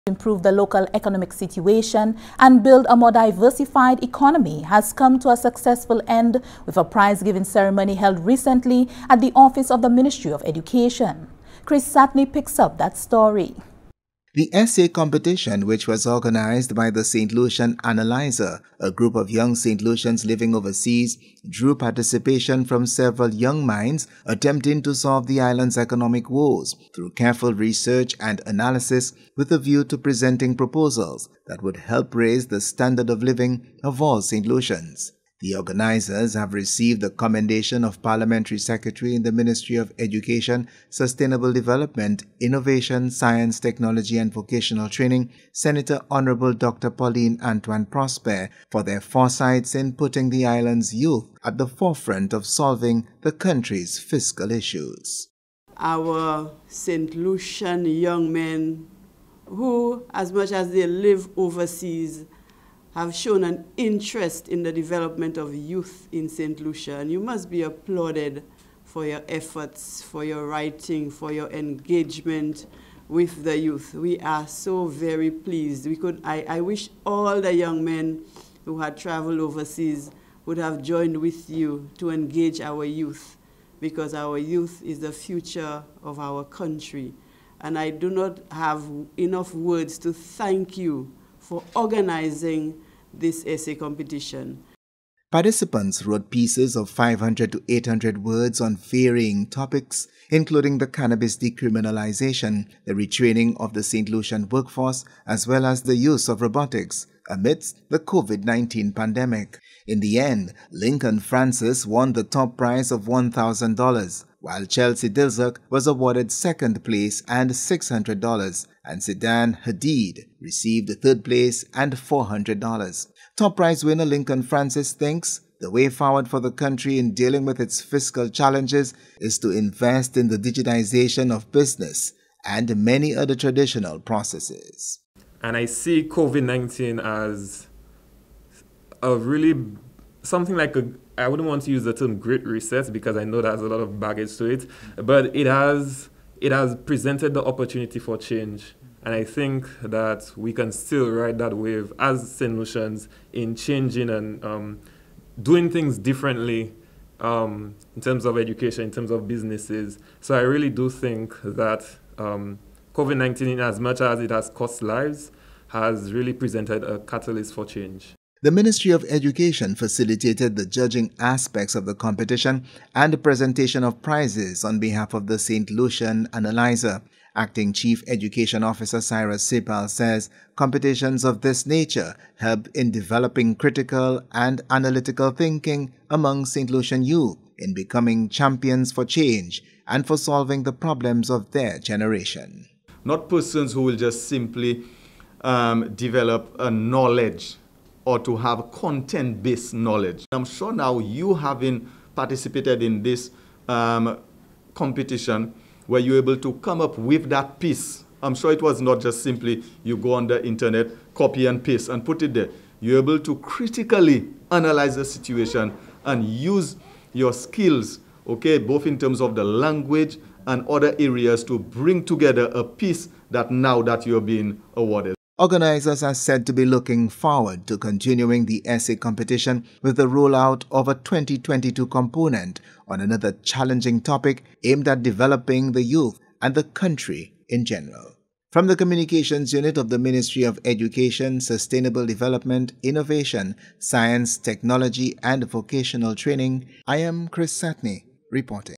Essay Competition Prize Giving Ceremony
An essay competition which challenged young Saint Lucians between the ages of 15 and 30 years to make suggestions to help improve the local economic situation and build a more diversified economy has come to a successful end with a prize giving ceremony held recently at the offices of the Ministry of Education.